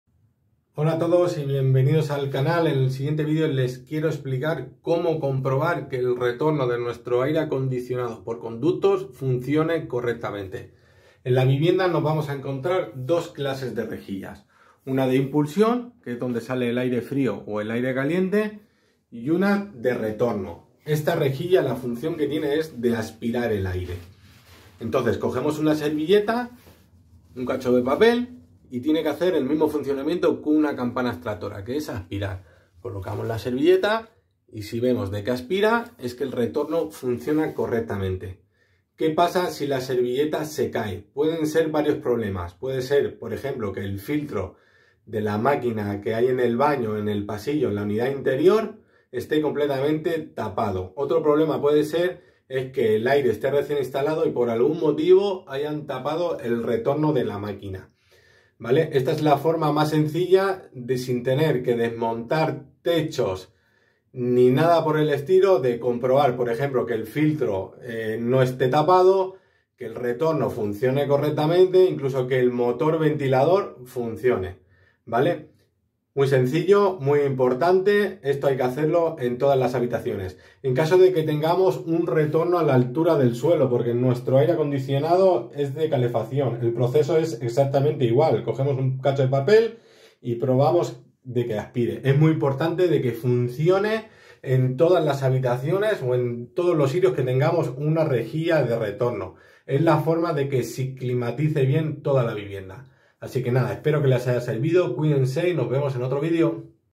Como comprobar si el retorno del aire por conductos funciona correctamente
Como comprobar si el retorno del aire acondicionado por conductos funciona correctamente